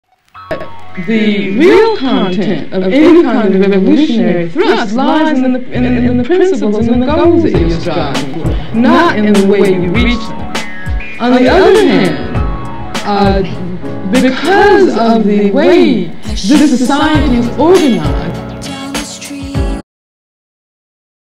Revolution and Violence / Angela Davis
Food footage Replicated video of Angela Davis explaining the path to revolution is in its ideas not how you get there. I am Black, made on the occasion to add my voice to protest the shame of hatred.